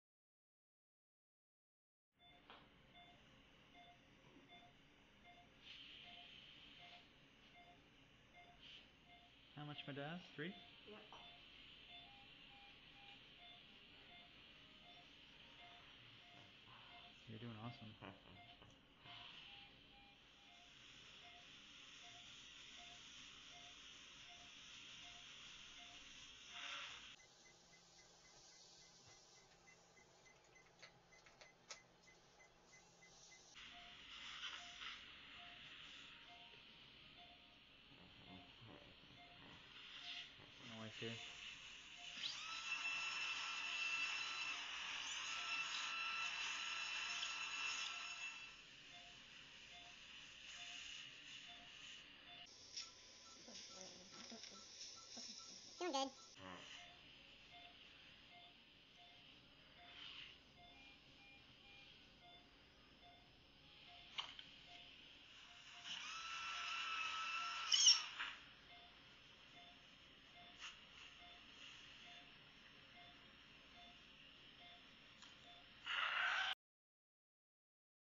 Tissue Impaction Wisdom Tooth Removal
Basic Tissue Impaction. If you watch a lot of my videos you see I don’t fight around with elevators and forceps for too long with teeth like this. It’s more efficient for me to trough and go to a purchase point if the tooth is being stubborn with elevators and forceps.

📌 I do try and get the tooth out with elevators and forceps first.

📌Then go to a buccal trough. If it still won’t pop out then try forceps..

📌 Purchase point for the win this time. If that didn’t work then I would section it.